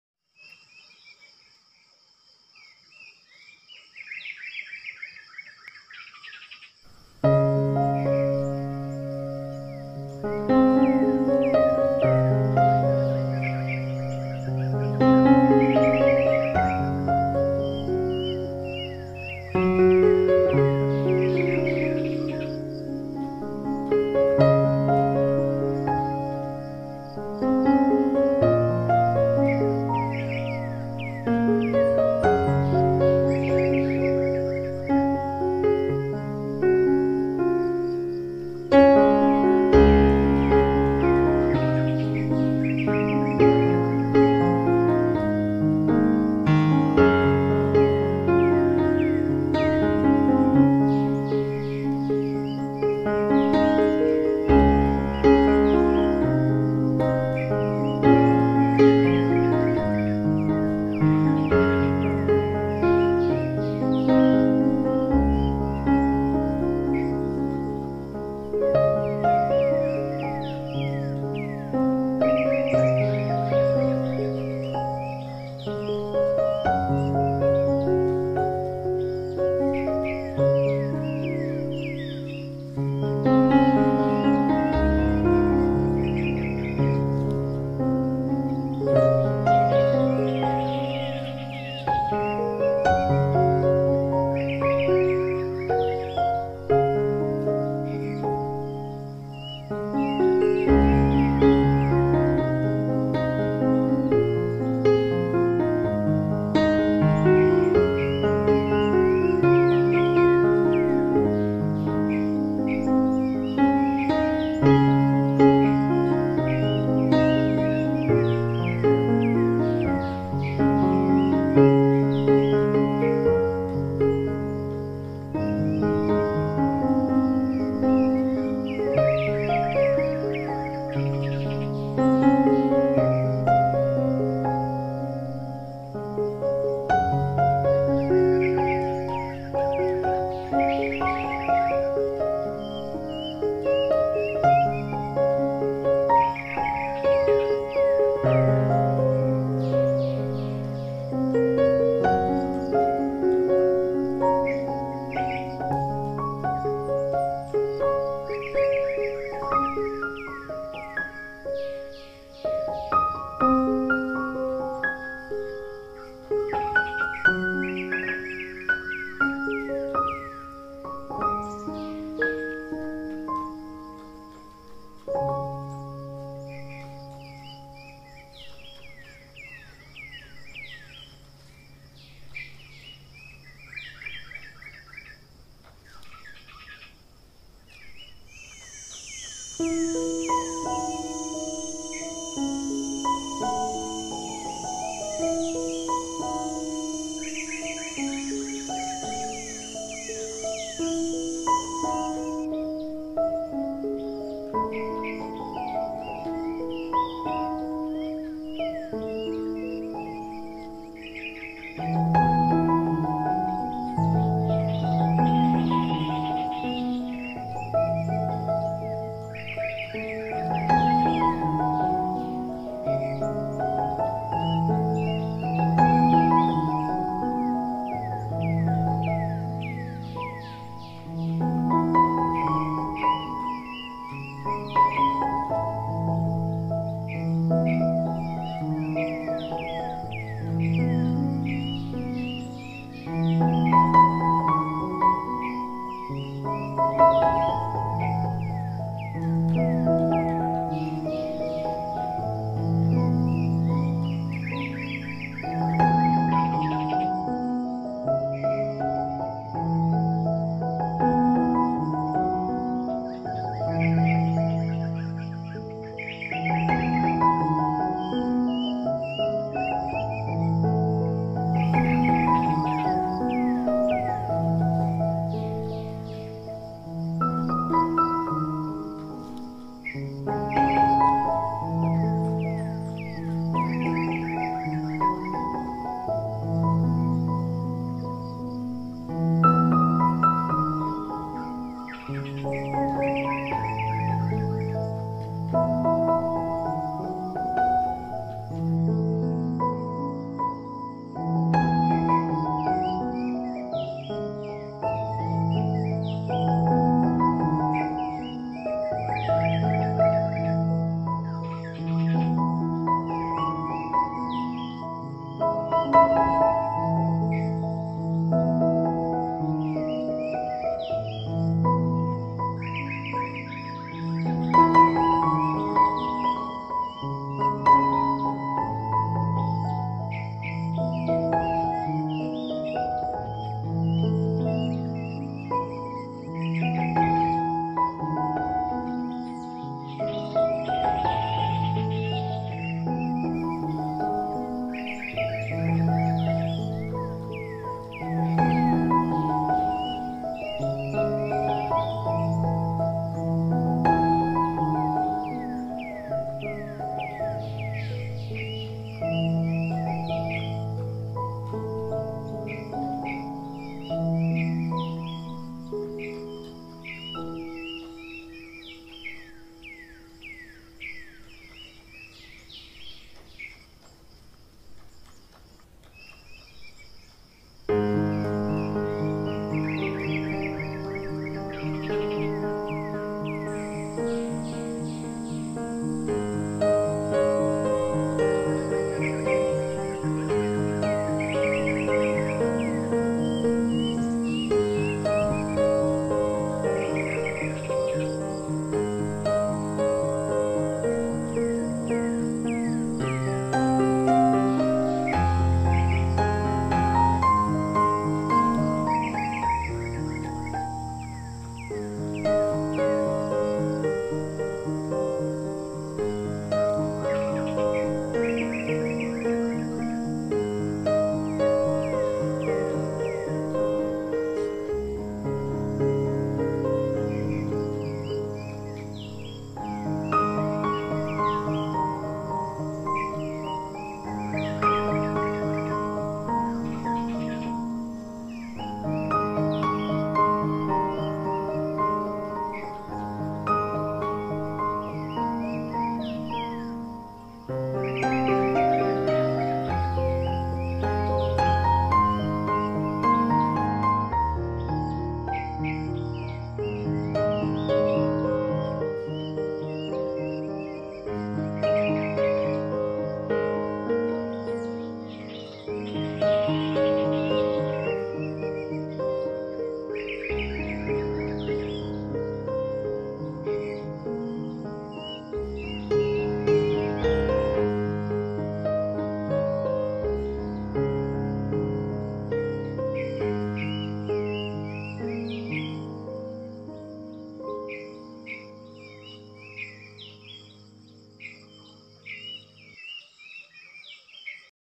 The Borneo Rainforest Mushrooms (4K HD)relaxing piano music & Birds Sounds#
All the pictures and videos about the wild mushrooms found in RainForest of Borneo were taken by myself.Its took four month to complete.

After carefully researching the pictures and vedios about The Borneo RainForest Mushrooms, then I combined all the videos and pictures into an eight-minute videos.

I hope you be able to enjoy it..

(These images and vedios were taken using a smartphone and a Canon EOS Camera)